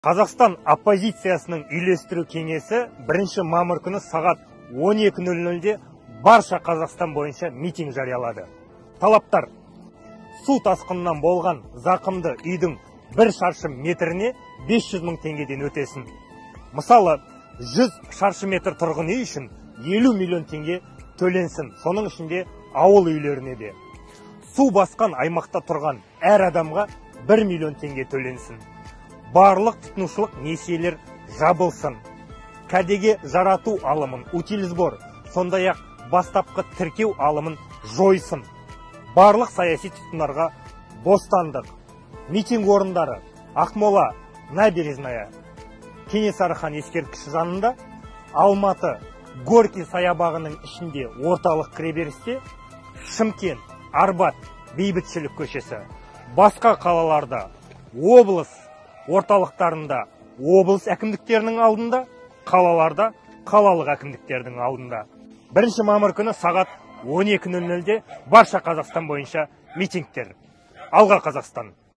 МИТИНГТЕР — 1 МАМЫР САҒАТ 12:00-ДЕ — БАРША ҚАЗАҚСТАН БОЙЫНША!
❗️ТАЛАПТАР❗️

1. СУ ТАСҚЫНЫНАН БОЛҒАН ЗАҚЫМДЫ — ҮЙДІҢ БІР ШАРШЫ МЕТРІНЕ 500 МЫҢ ТЕҢГЕДЕН ӨТЕСІН.

❗️Мысалы, 100 шаршы метр тұрғын үй үшін 50 миллион теңге төленсін.
(СОНЫҢ ІШІНДЕ АУЫЛ ҮЙЛЕРІНЕ ДЕ!)

2. СУ БАСҚАН АЙМАҚТА ТҰРҒАН ӘР АДАМҒА — 1 (БіР) МИЛЛИОН ТЕҢГЕ ТӨЛЕНСІН

3. КӘДЕГЕ ЖАРАТУ АЛЫМЫ (УТИЛЬСБОР) ЖӘНЕ БАСТАПҚЫ ТІРКЕУ АЛЫМЫ ЖОЙЫЛСЫН

4. БАРЛЫҚ ТҰТЫНУШЫЛЫҚ НЕСИЕЛЕР ЖАБЫЛСЫН

5. САЯСИ ТҰТҚЫНДАРҒА БОСТАНДЫҚ

📍МИТИНГ ОРЫНДАРЫ📍

🔵 АҚМОЛА — Набережная. Кенесары Хан ескерткіші

🔵 АЛМАТЫ — Горький саябағының ішінде, орталық кіреберісте, Гоголь көш.

🔵 ШЫМКЕНТ — Арбат, Бейбітшілік көш.

❗️БАСҚА ҚАЛАЛАРДА❗️

 📍Облыс орталықтарында - облыс әкімдіктерінің алдында.

 📍 Қалаларда – қалалық әкімдіктердің алдында.

ЗАҚЫМДЫ ӨТЕУГЕ 50 МИЛЛИОН ТЕҢГЕ ТӨЛЕНСІН!